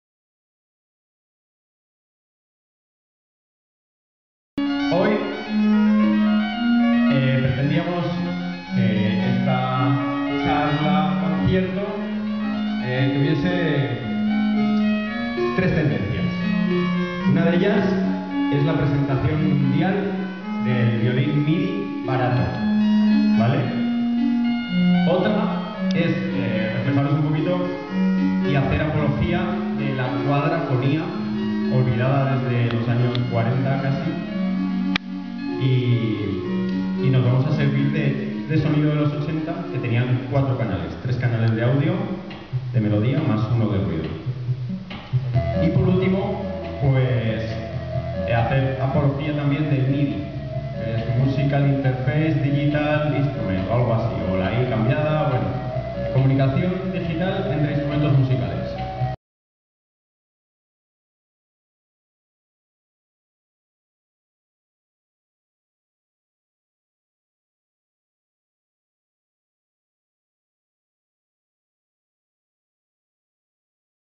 RetroMañía'09: el Retroconciertazo - Presentación
Presentación de El Retroconciertazo, actividad celebrada dentro del evento RetroMañía'09, que tuvo la oportunidad de presentar al público el flamante violín MIDI recién fabricado artesanalmente por el propio Jaime "Rockriver" Lapeña.